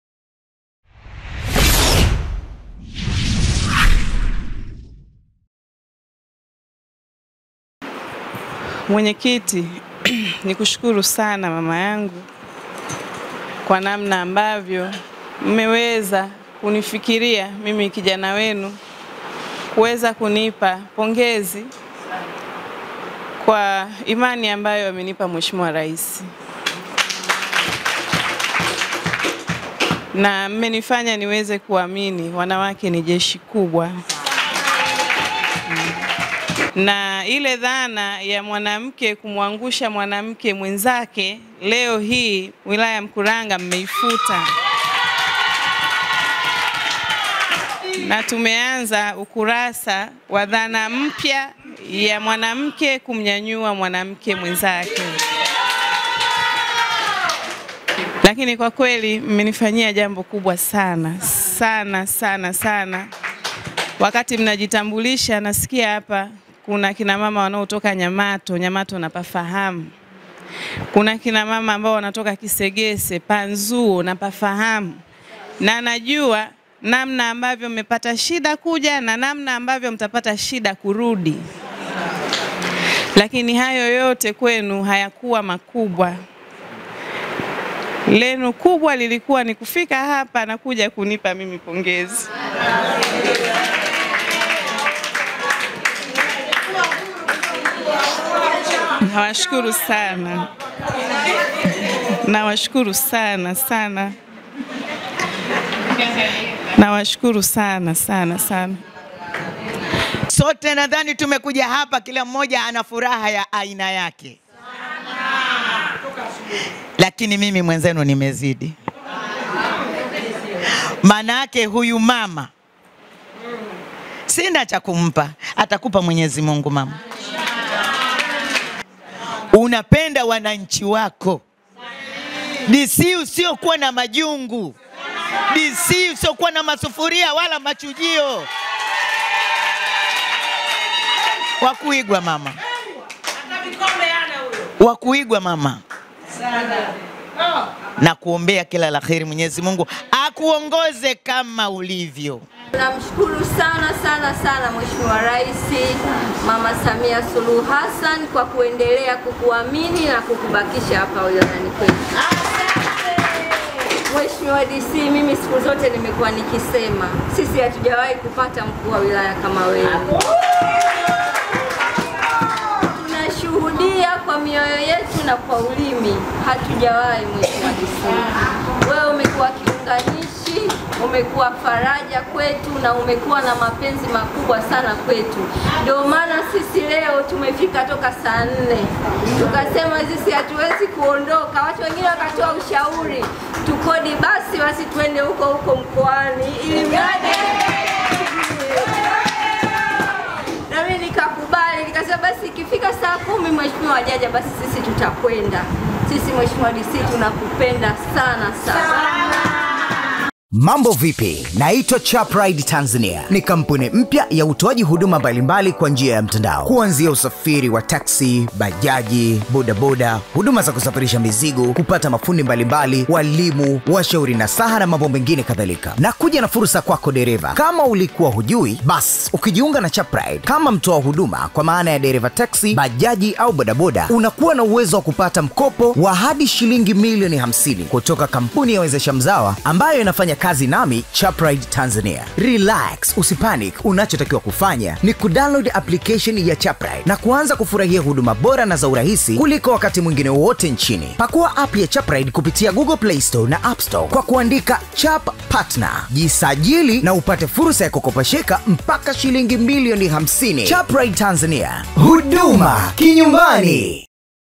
DC ASHINDWA KUJIZUIA AMWAGA MACHOZI HADHARANI, UWT MKURANGA WAMPONGEZA KWA KAZI NZURI...
Umoja wa wanawake wilaya ya Mkuranga wamempongeza mkuu wa wilaya hiyo Bi Khadija Nasir  kwa kazi nzuri na kuaminiwa na Mhe.Rais kuendelea kuwatumikia wana Mkuranga.